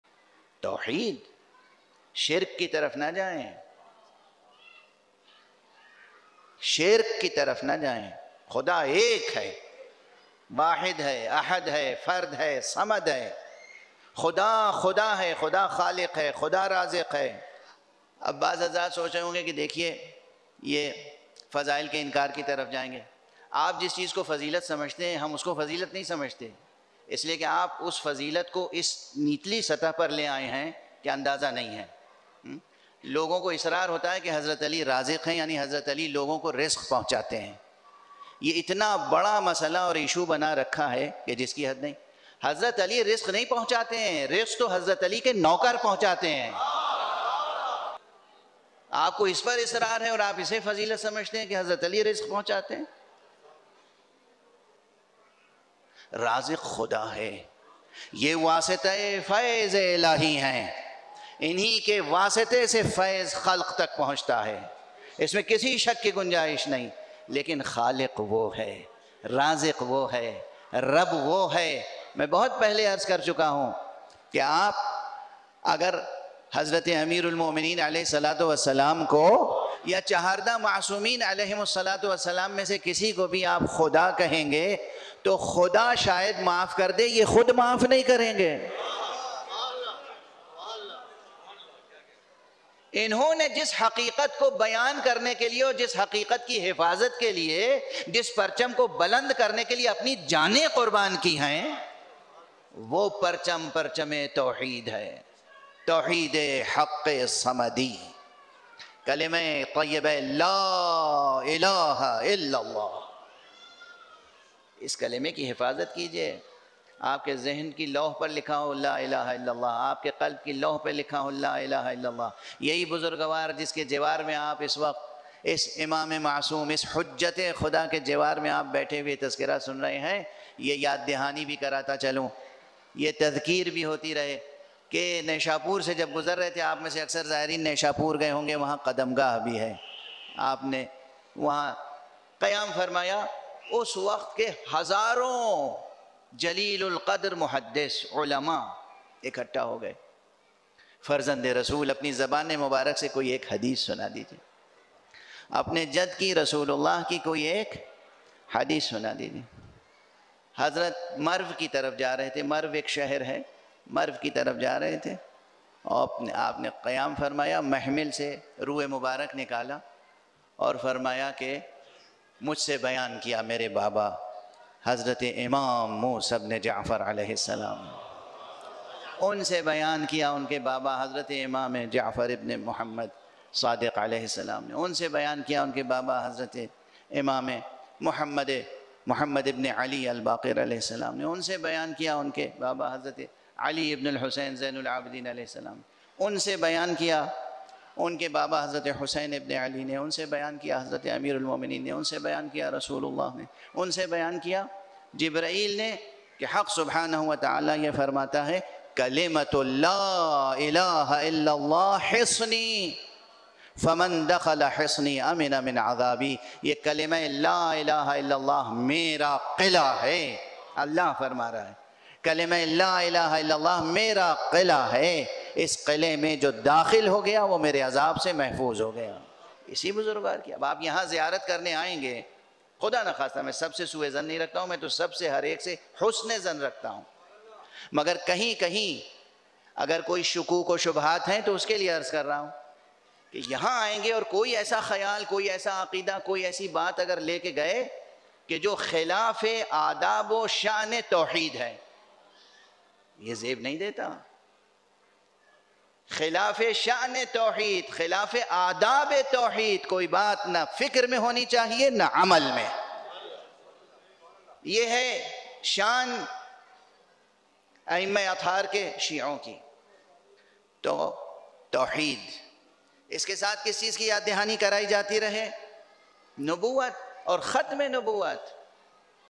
Kya Mola Ali Rizaq Takseem Krtay Hain ? | Allama Syed Aqeel ul Gharvi
Kya Mola Ali Rizaq Takseem Krtay Hain ? | Allama Syed Aqeel ul Gharvi | Al hadi islamic Center.